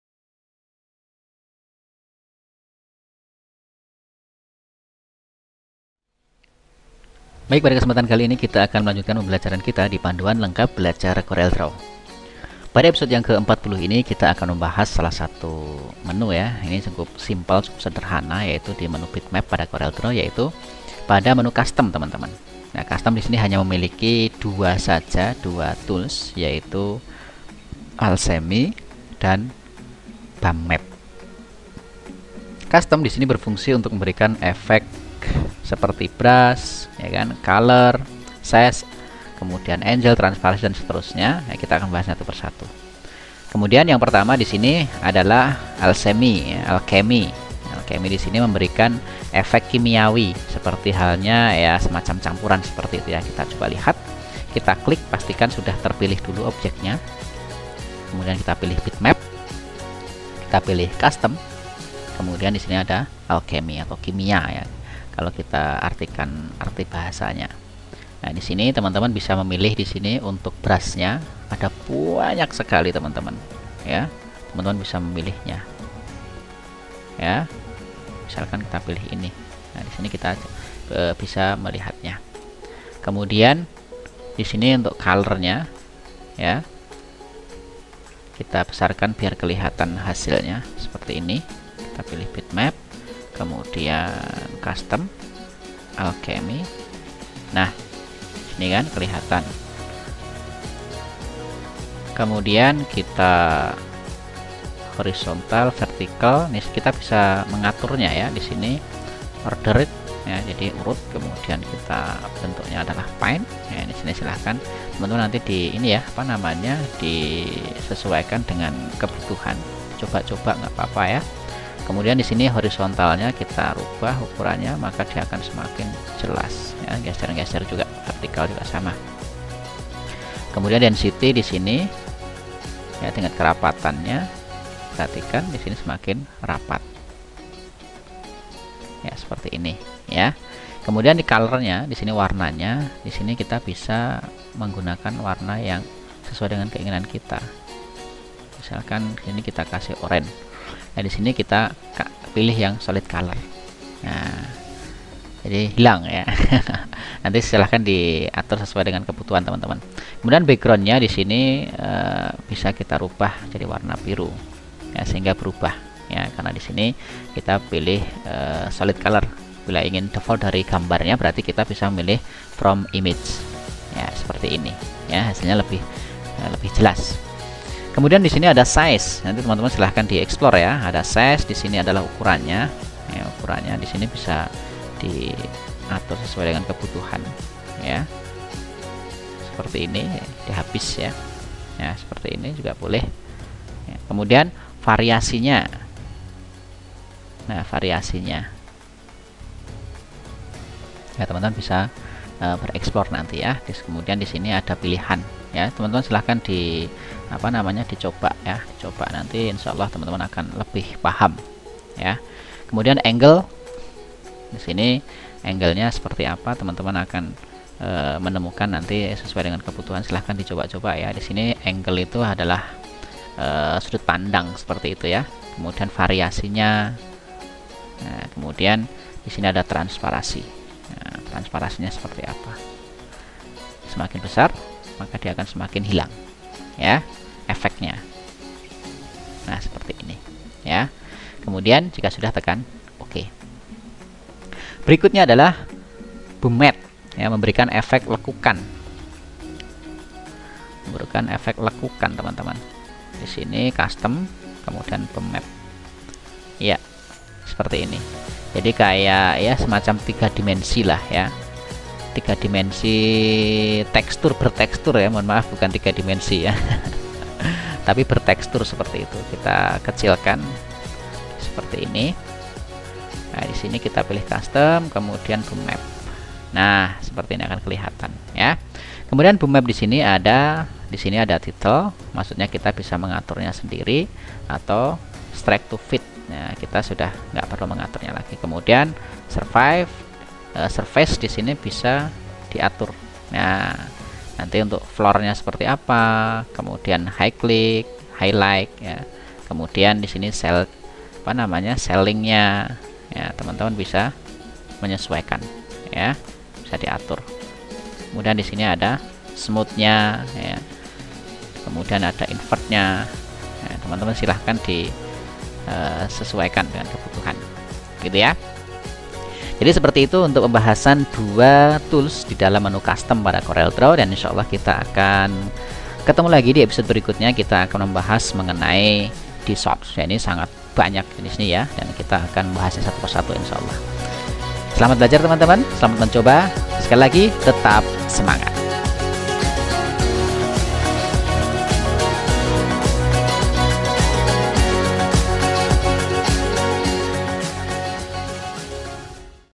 Tutorial CorelDraw Dasar #40 Custom
Pada tutorial corelDraw dasar bagian ke 40 ini kita membahas tentang menu Custom pada coreldraw.

Selamat mengikuti!

CUSTOM
Berfungsi untuk memberikan efek-efek seperti brush, color, size, angle, transparency, bump map, surface, dan lighting

1. Alchemy (memberikan efek kimiawi)
2. Bum-Map (memberikan efek lekukan)

Bagikan video ini sebanyak-banyaknya dan jika ada yang kurang jelas atau masukan bisa dituliskan dikolom komentar.